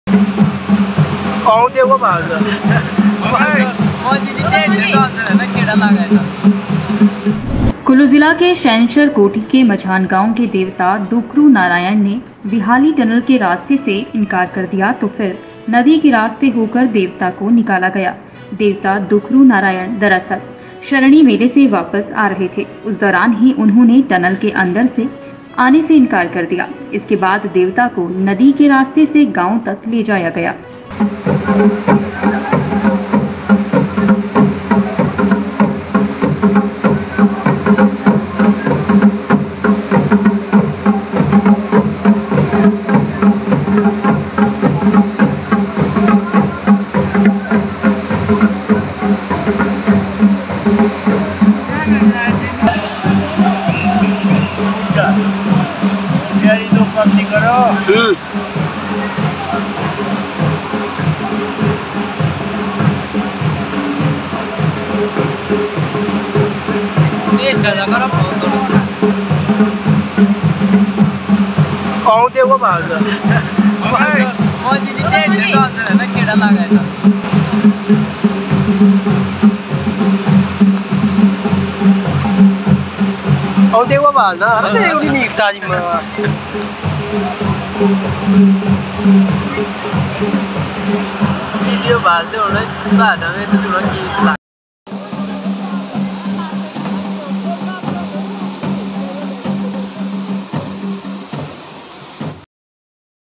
Sainj | Dukhru Narayan Devta
Sainj (Kullu) :  When the deity Dukhru Narayan of Majhan village of Shenshar Kothi in Kullu district refused to come through the Vihali tunnel, the deity was taken out through the river. Actually, the deity Dukhru Narayan was returning home from the Sharani fair, during which time he refused to come through the tunnel. After this, the deity was taken to the village through the river.